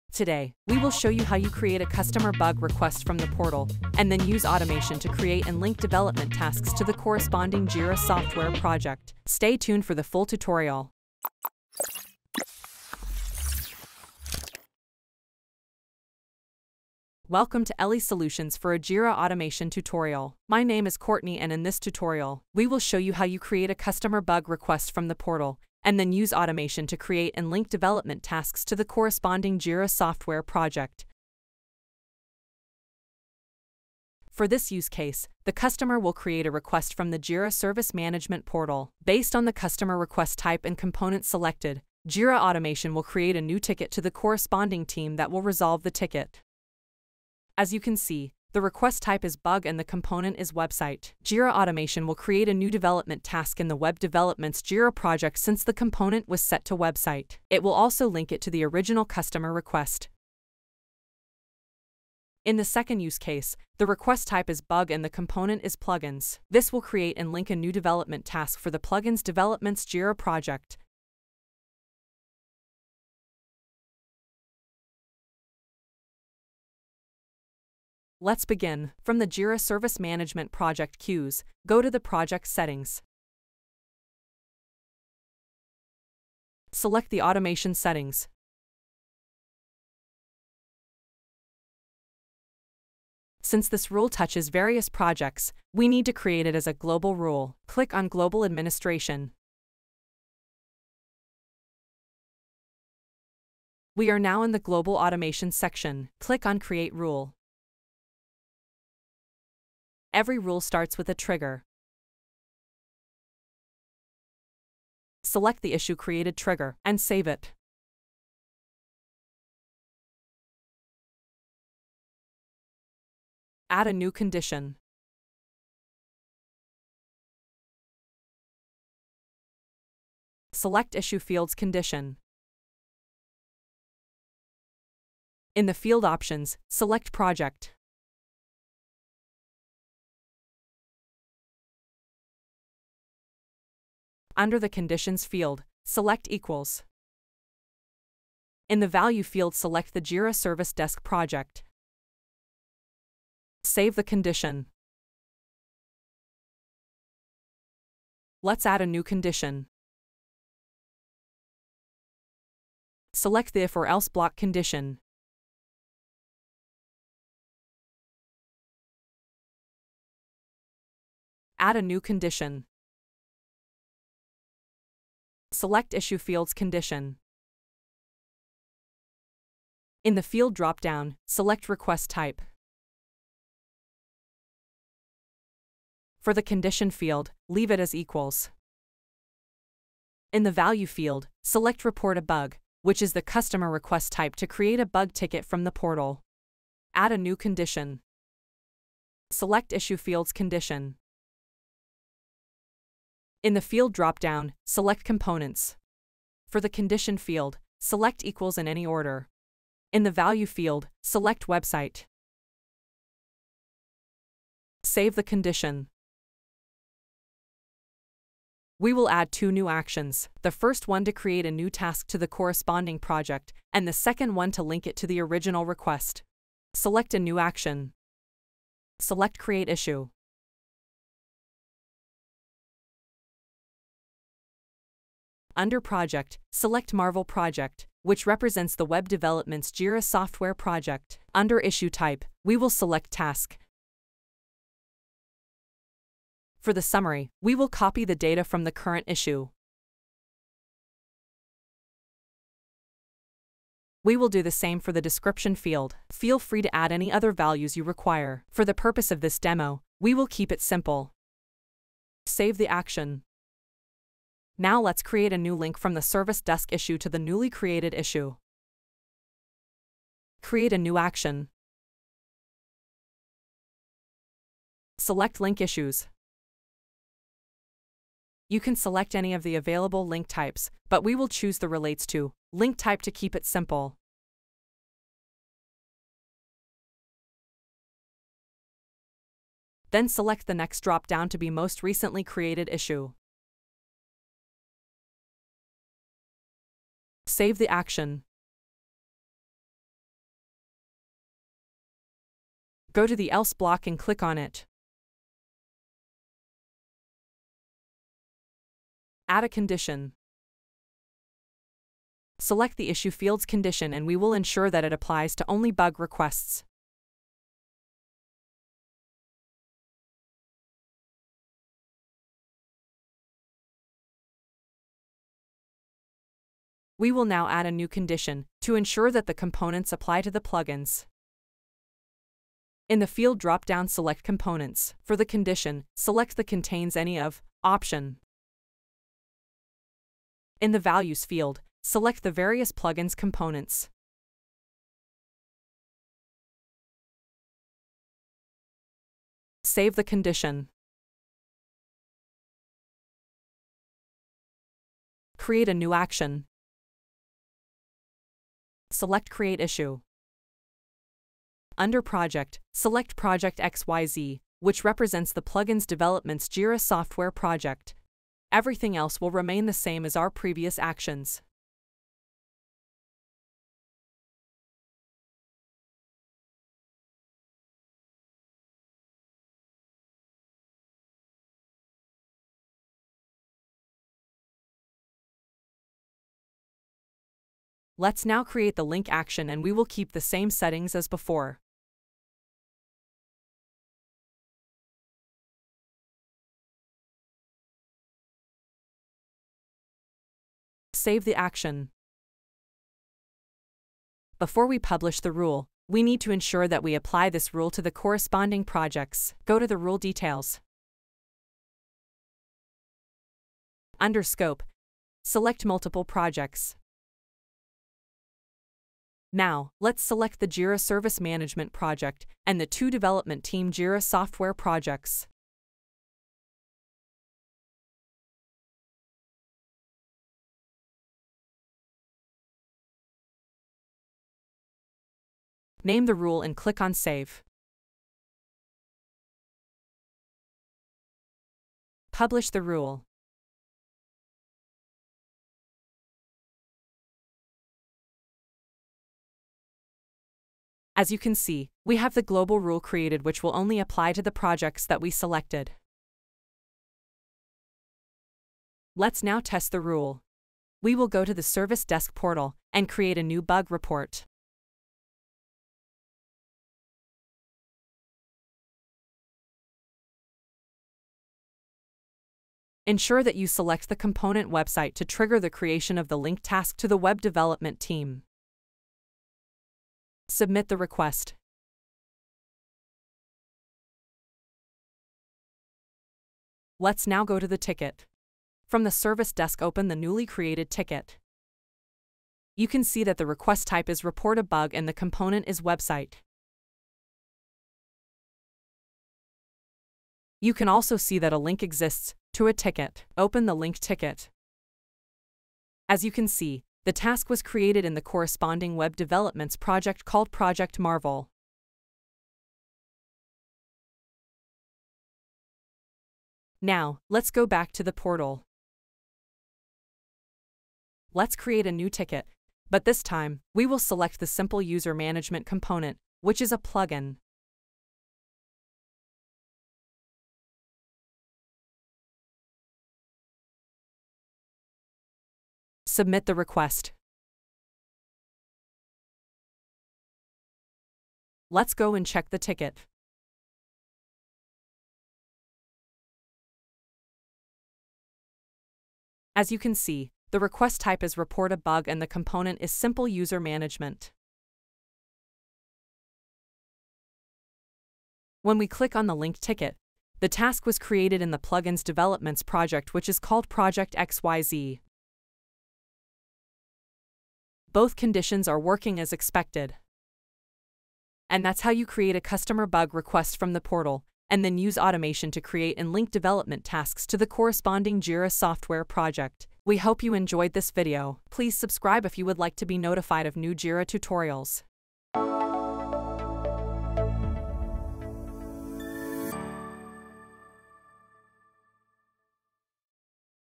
Jira Automation: JSM JS Synchronization: Create Tasks
In this tutorial we will show you how you create a customer bug request from the portal and then use automation to create and link development tasks to the corresponding jira software project.

Summary:
00:00 Introduction
01:18 Set Issue Created Trigger
02:03 Configure Project Condition
02:36 Configure if/else block Condition 1: Create and link task for Web Development Team
05:02 Configure if/else block Condition 2: Create and link task for Plugins Development Team
06:50 Rule Details - Configure Multiple Projects
07:38 Rule Validation